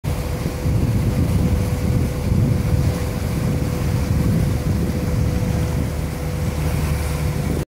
Dolphins in Eagle Texas' bow wave
As the motor tanker Eagle Texas steams out of Corpus Christi some dolphins play in its bow wave.